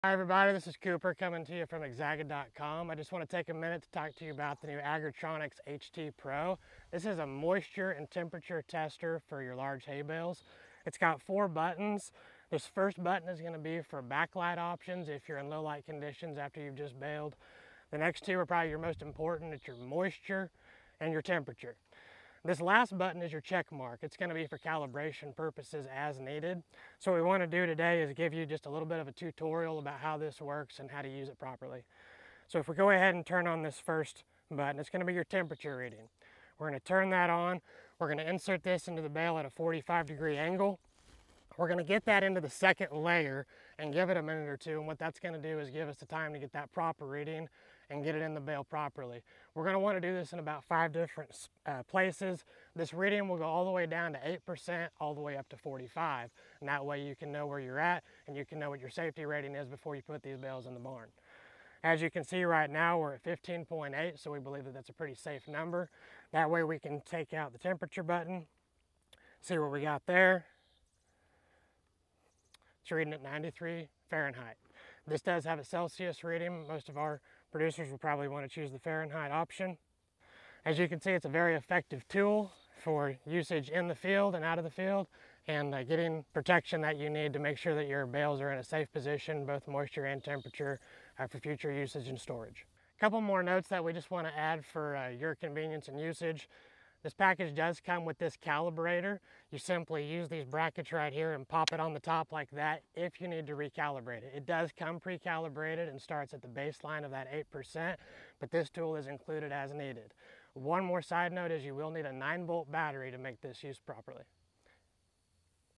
How to Use the HT-PRO Hay Moisture Tester (Full Demo + Tips)
Learn how to properly use, test, and calibrate the HT-PRO Hay Moisture Tester with 20-inch Probe by AgraTronix!

In this video, we walk you through each of the 4 buttons, explain what they do, and show you how to perform real-time moisture and temperature testing in your baled hay. Plus, we demo a live test so you can see the results in action 🌾

Designed for farmers, ranchers, and hay producers, the HT-PRO is the go-to tool for ensuring proper hay storage and preventing spoilage or fire risk.

🔧 What You'll Learn in This Video:
-How to use each button on the tester
-How to calibrate with the included clip
-Live testing demo on baled hay
-How to get accurate moisture & temp readings

🛠️ Features Include:
✅ 20” stainless steel probe
✅ Live LCD display
✅ Reads moisture (8%–44%) & temp (32°F–225°F)
✅ Battery powered (9V) for field use

Avoid hay loss and preserve quality with real-time hay testing done right.